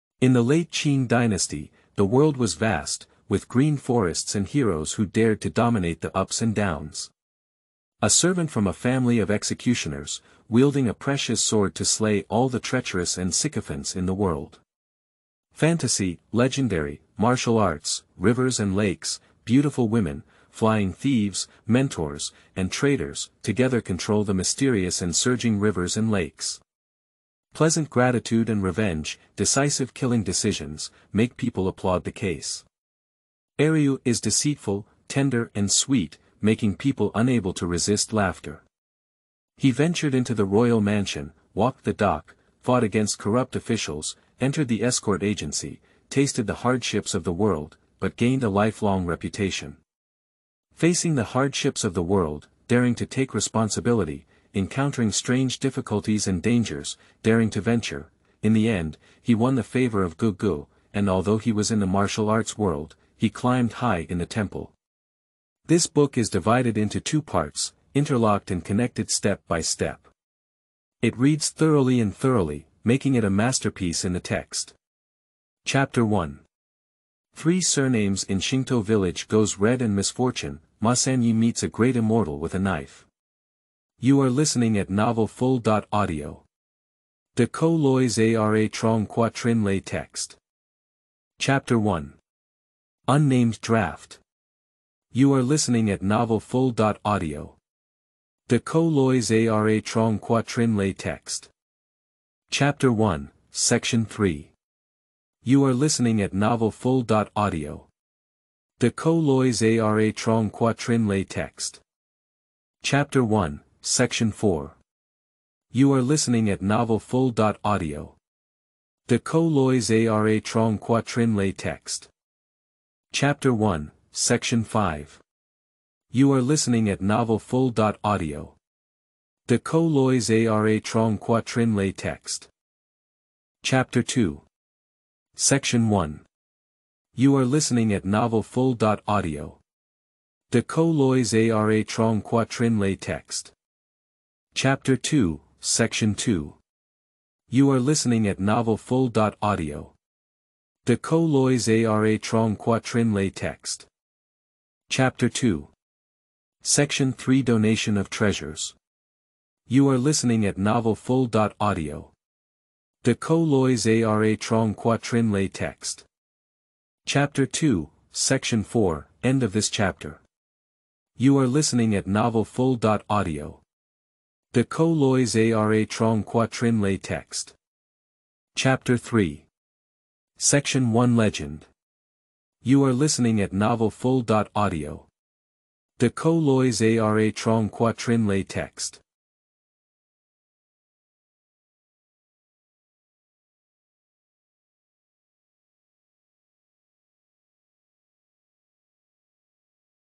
Guillotine Knife EP1-10 FULL | 断头刀
00:00:00 Chapter 1: Three Surnames in Xingtou Village Goes Red and Misfortune, Ma San Ye Meets a Great Immortal with a Knife
00:01:31 Chapter 1: Unnamed Draft
00:01:42 Chapter 1:, Section 3
00:01:52 Chapter 1:, Section 4
00:02:02 Chapter 1:, Section 5
00:02:13 Chapter 2: Section 1
00:02:24 Chapter 2:, Section 2
00:02:34 Chapter 2: Section 3 Donation of Treasures
00:02:46 Chapter 2:, Section 4, End of this chapter
00:02:58 Chapter 3: Section 1 Legend

Intro:

In the late Qing Dynasty, the world was vast, with green forests and heroes who dared to dominate the ups and downs. A servant from a family of executioners, wielding a precious sword to slay all the treacherous and sycophants in the world. Fantasy, legendary, martial arts, rivers and lakes, beautiful women, flying thieves, mentors, and traitors, together control the mysterious and surging rivers and lakes. Pleasant gratitude and revenge, decisive killing decisions, make people applaud the case. Eryu is deceitful, tender and sweet, making people unable to resist laughter. He ventured into the royal mansion, walked the dock, fought against corrupt officials, entered the escort agency, tasted the hardships of the world, but gained a lifelong reputation. Facing the hardships of the world, daring to take responsibility, encountering strange difficulties and dangers, daring to venture; In the end, he won the favor of Ge Ge, and although he was in the martial arts world, he climbed high in the temple. This book is divided into two parts, interlocked and connected step by step. It reads thoroughly and thoroughly, making it a masterpiece in the text.

You are listening Guillotine Knife Episode 1 chapter 1 to 10